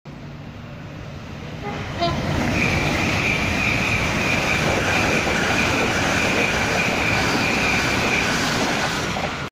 Two Class 387 Electrostars and a Class 800 IET at Speed through Acton Main Line
One of the best stations for trainspotting, at Acton Main Line there's such a vast array of trains its a fantastic place to film trains. More videos from Acton Main Line to come over the next few days.